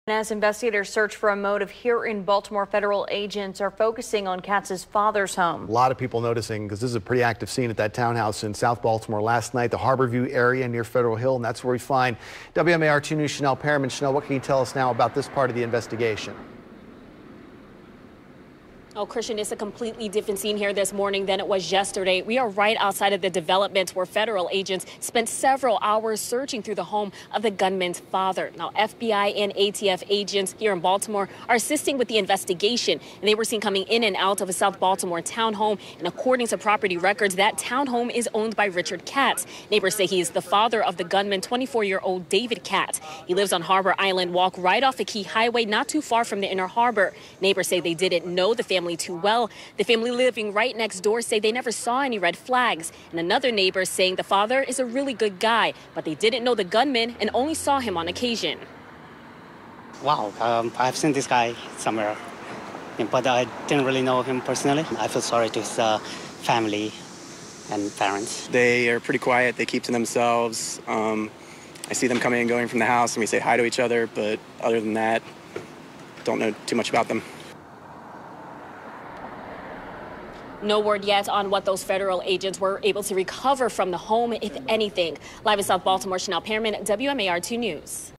Suspected shooter David Katz from Baltimore
"It’s a complete shock."  The Jacksonville Sheriff’s Office believe the suspect in today’s video game tournament mass shooting is 24-year-old David Katz from Baltimore.